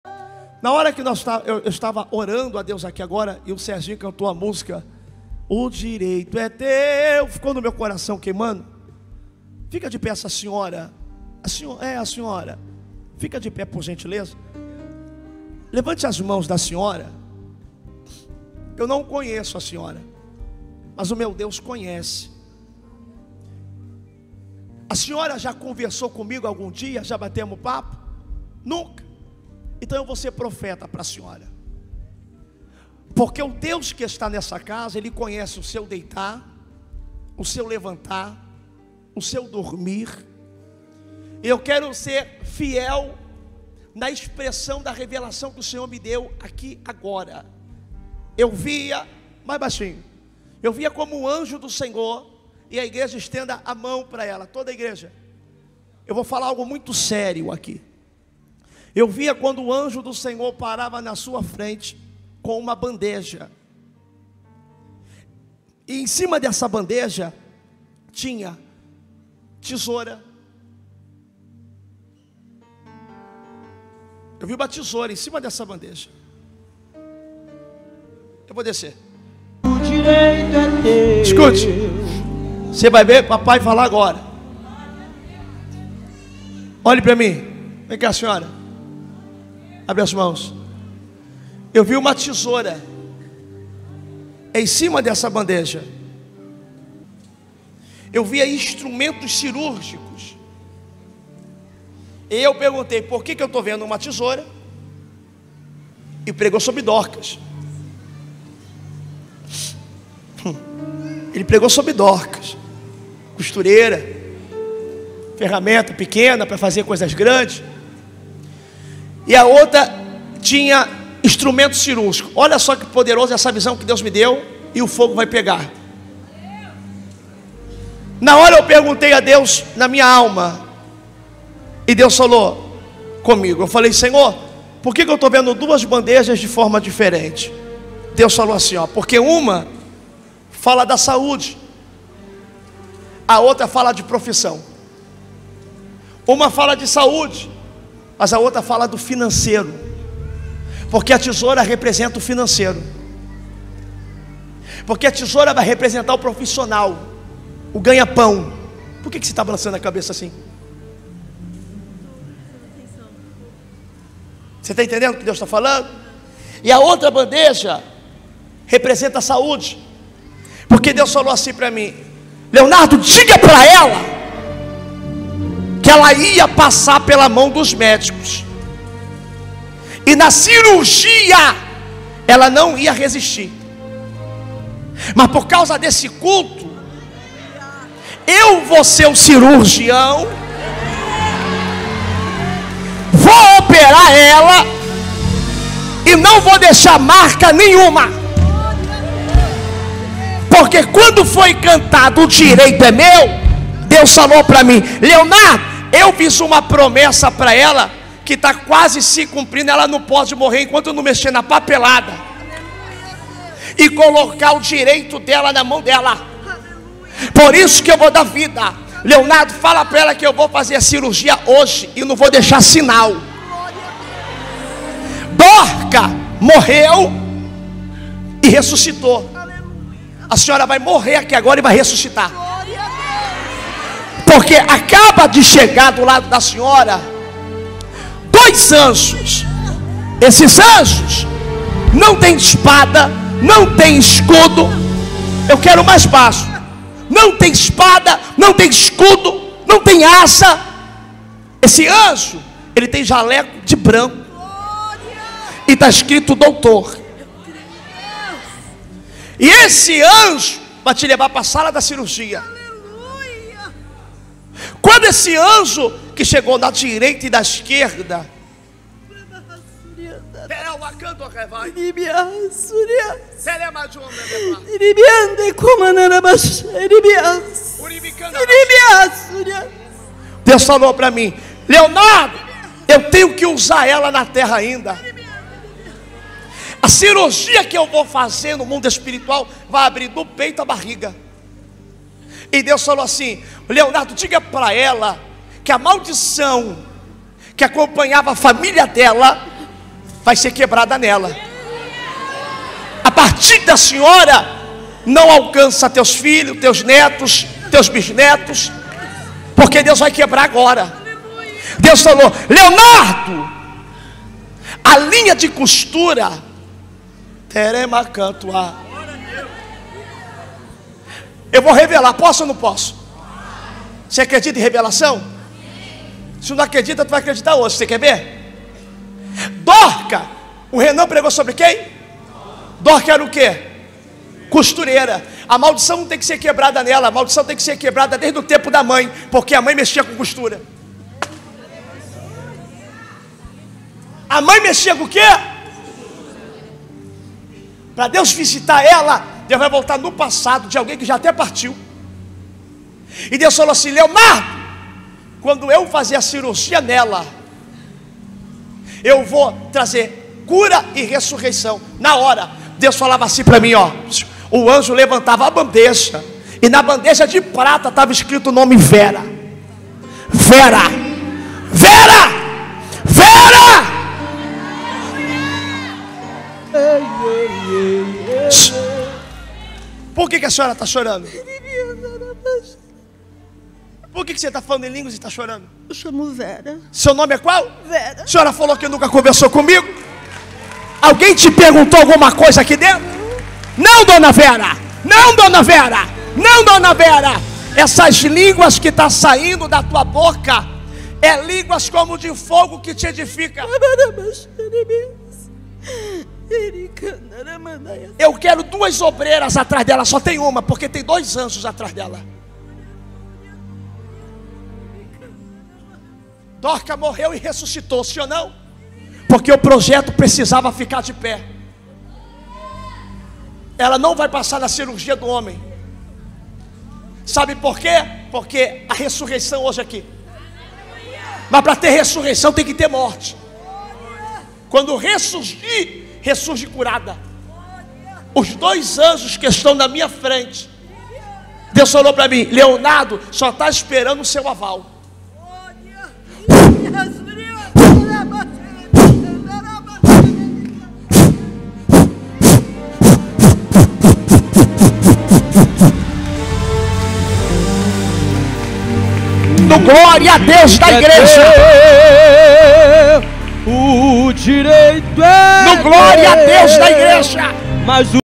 DEUS VAI MEXER NA PAPELADA E COLOCAR O DIREITO NA MÃO DELA - Pr Leonardo Sale
Forte Revelação com Pastor Leonardo Sale!
Aconteceu na Catedral IPTM Caxias no dia 29 de março de 2023.

Banco Bradesco:
Ag: 7049 - CC: 15762-7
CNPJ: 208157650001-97